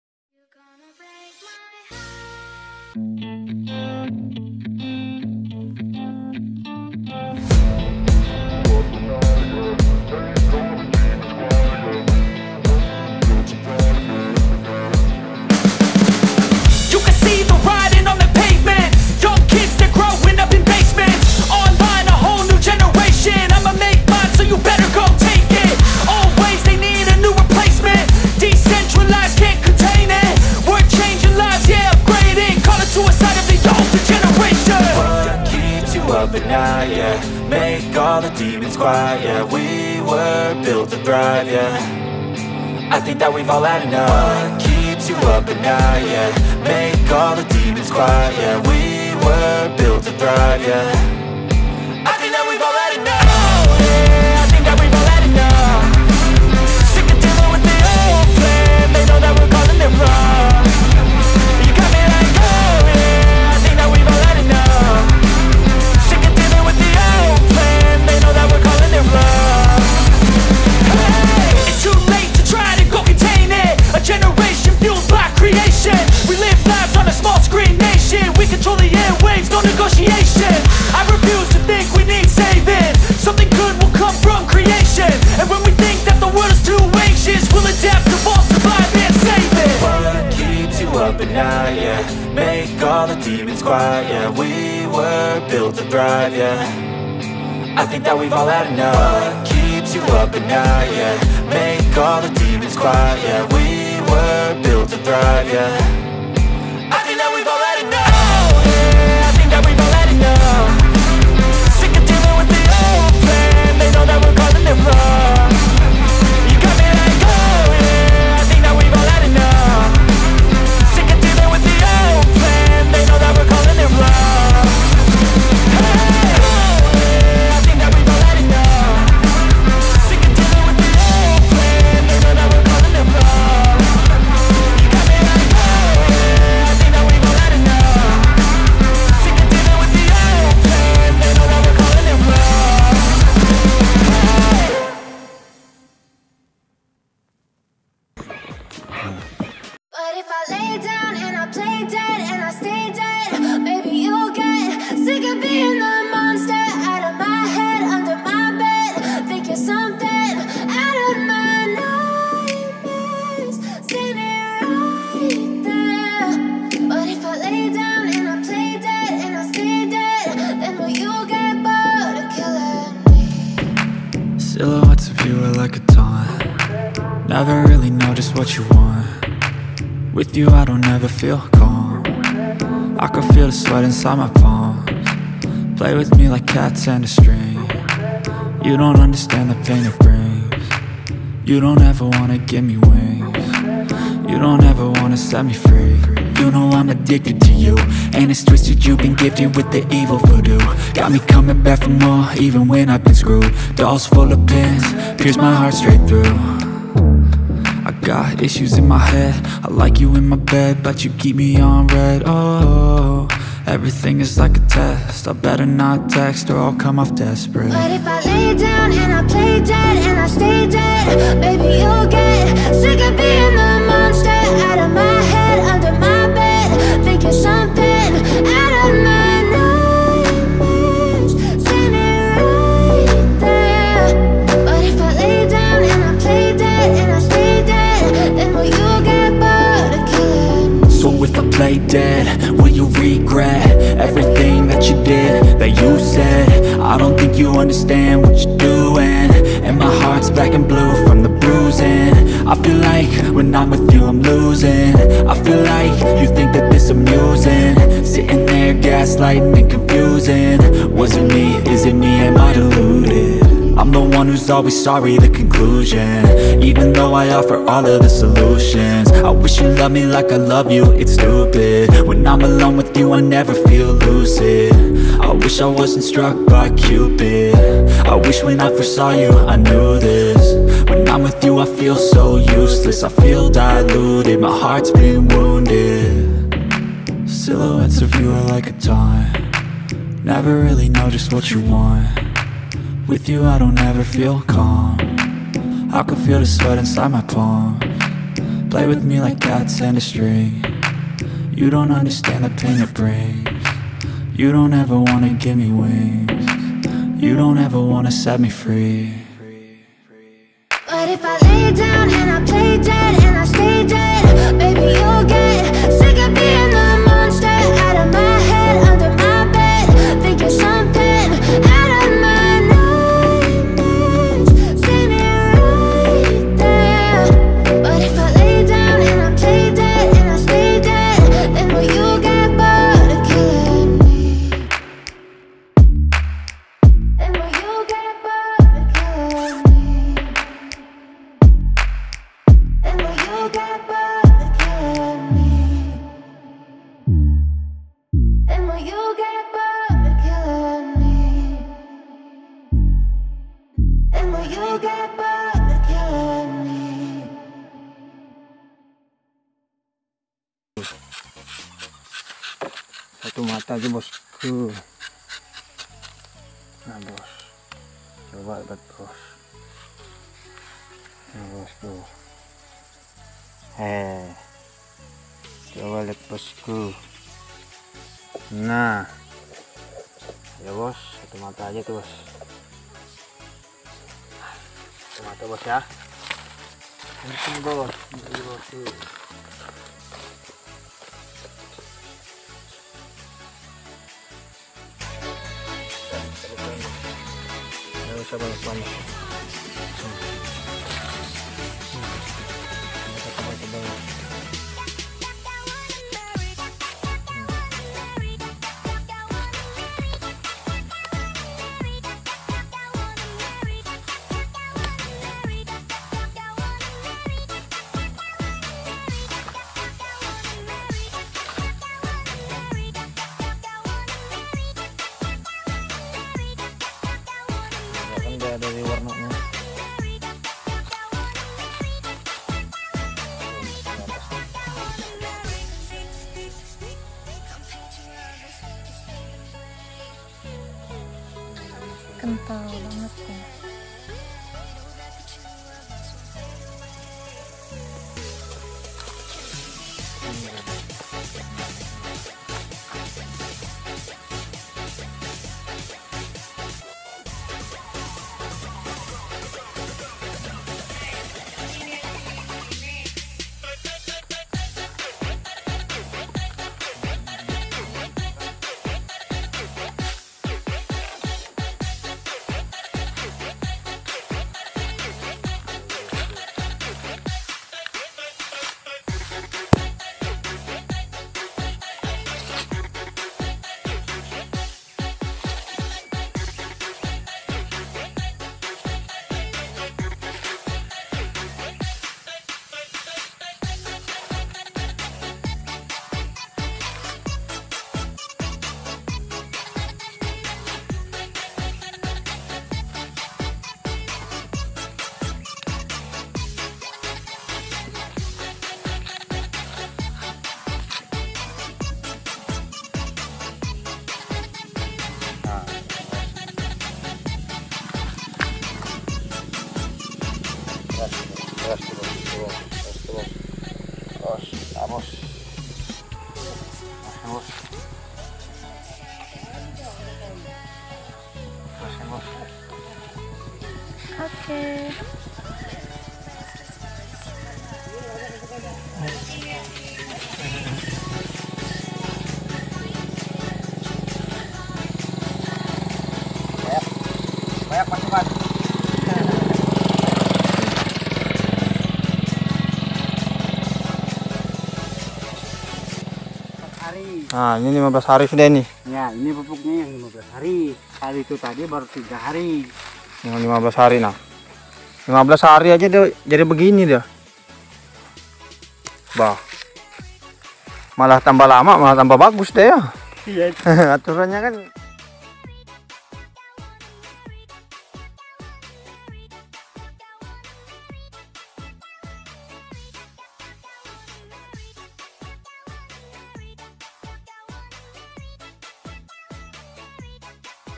Aplikasi Pupuk Organik NASA Untuk Tanaman Karet Kecamatan MuaraKomam Paser KALTIM
Paket lengkap pupuk organik NASA. POC NASA, HORMONIK, SPRK, dan AERO Perekat.